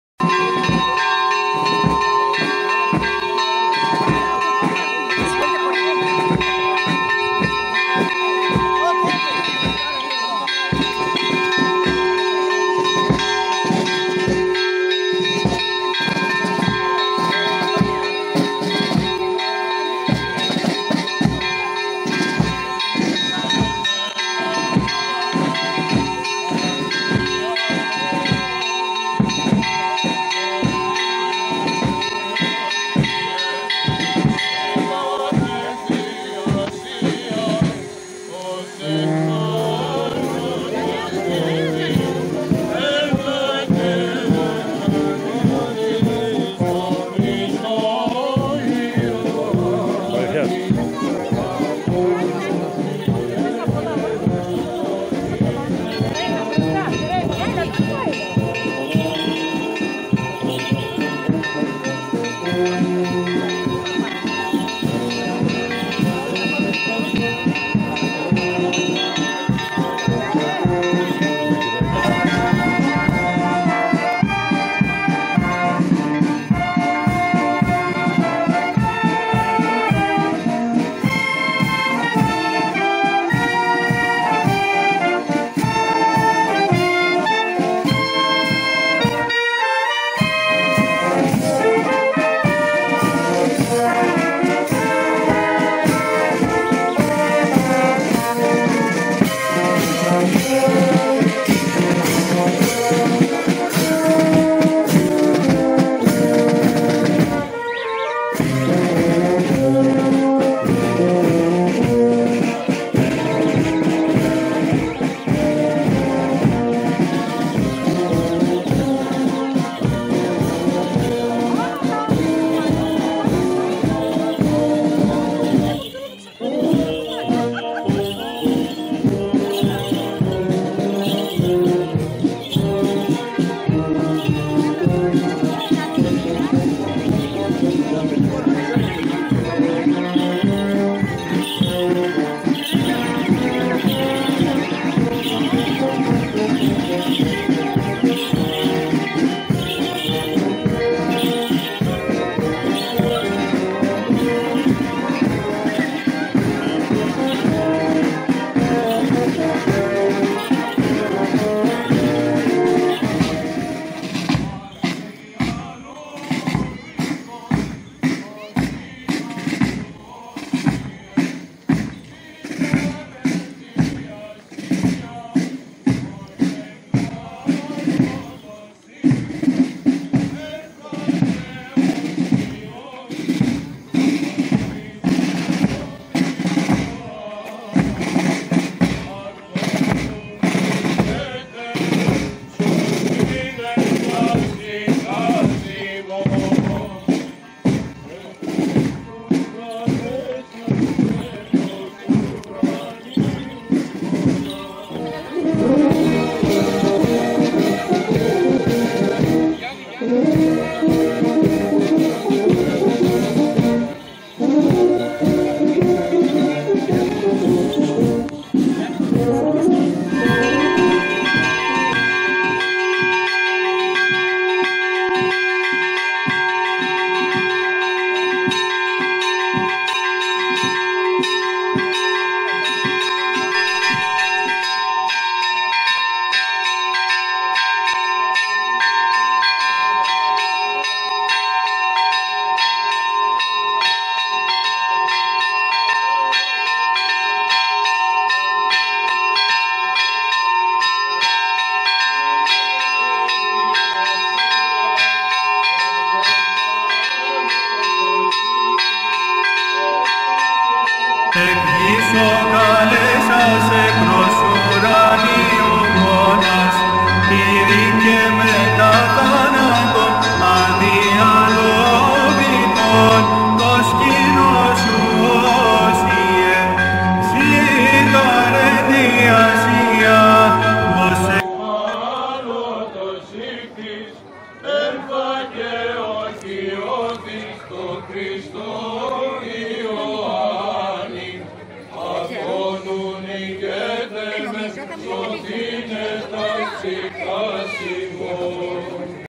ΛΙΤΑΝΕΥΣΗ ΤΗΣ ΙΕΡΑΣ ΕΙΚΟΝΟΣ ΤΟΥ ΑΓΙΟΥ ΙΩΑΝΝΟΥ ΤΟΥ ΡΩΣΣΟΥ ΛΑΥΡΙΟΥ 26 5 2022
ΠΡΩΤΟΣΤΑΤΟΥΝΤΟΣ ΤΟΥ ΣΕΒΑΣΜΙΩΤΑΤΟΥ ΜΗΤΡΟΠΟΛΙΤΟΥ
ΜΕΣΟΓΑΙΑΣ & ΛΑΥΡΕΩΤΙΚΗΣ
Κ.κ. ΝΙΚΟΛΑΟΥ